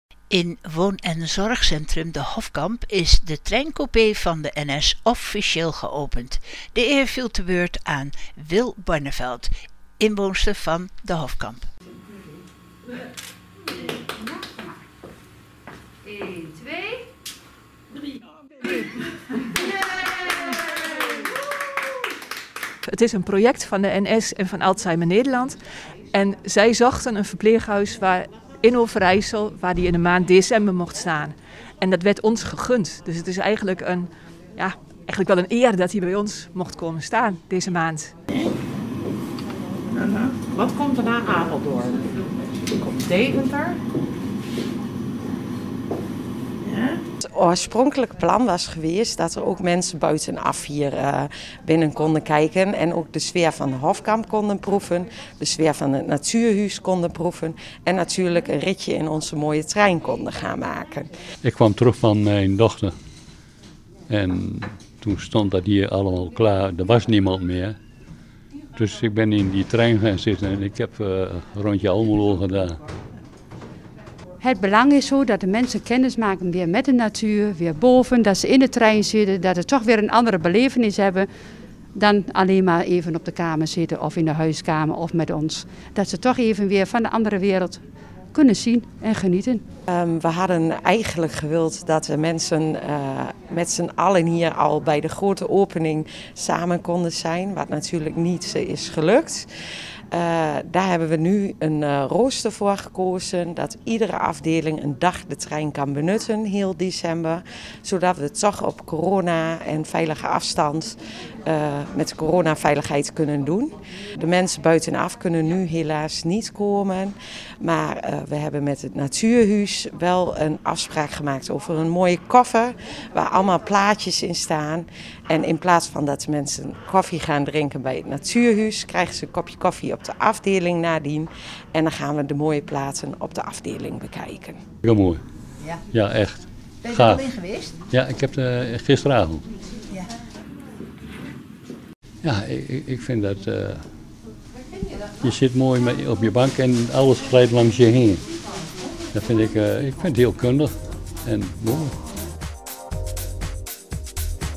Treincoupé in de Hofkamp geopend
Toch een feestje, ondanks de Corona. Mevrouw Wil Barneveld was de eer vergund de treincoupé van de NS  in de Hofkamp te openen.